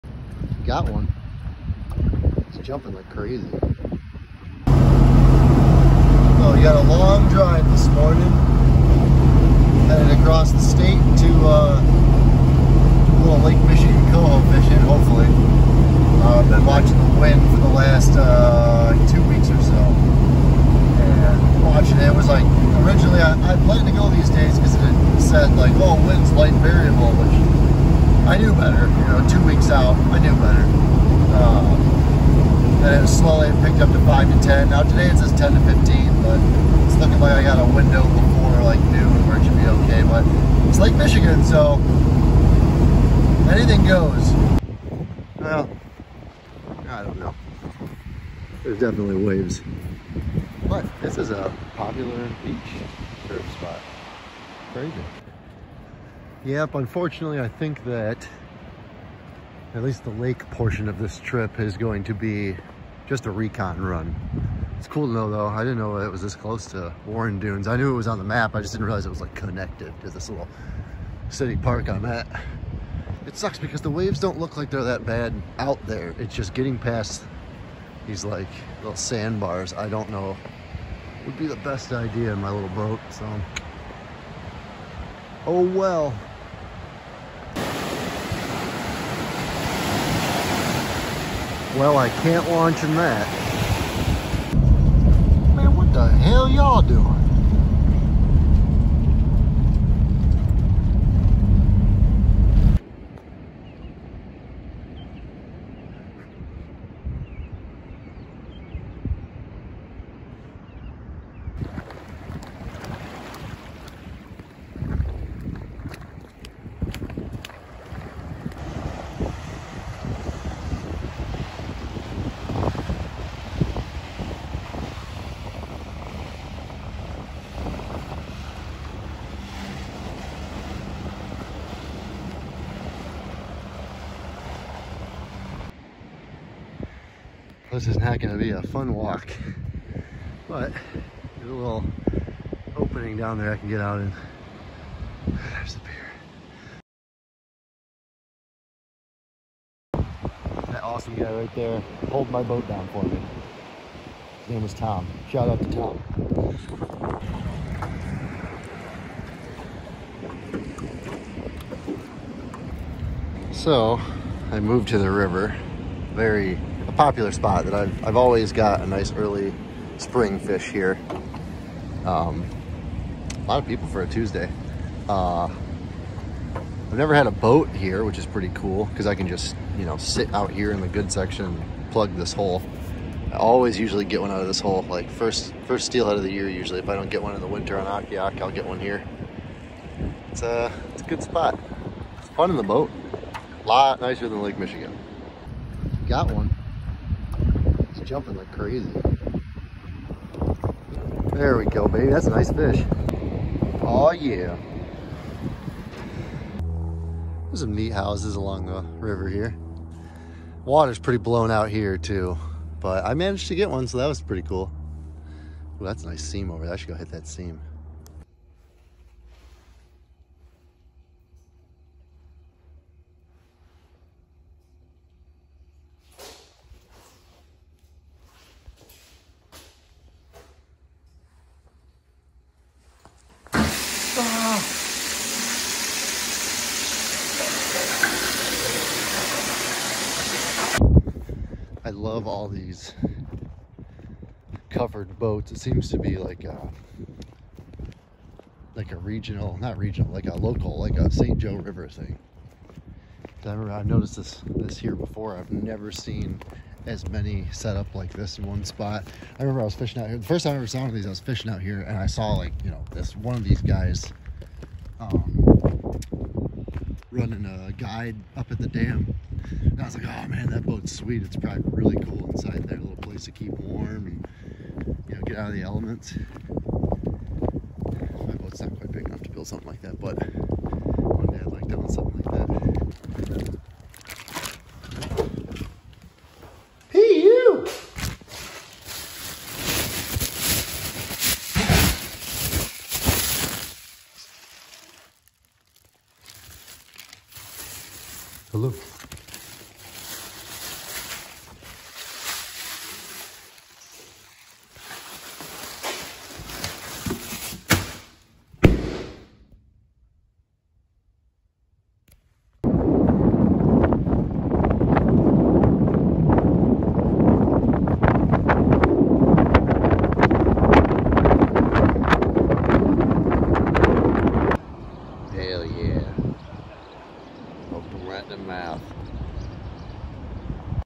Tiny boat Lake Michigan coho attempt & river pluggin’ for steelhead
My first attempt at running the big water NOT in a bay. Learned a lot. Waves as big as buicks. Shifted focus to some river plugging and picked up a nice hen. Time for a bigger boat, eh?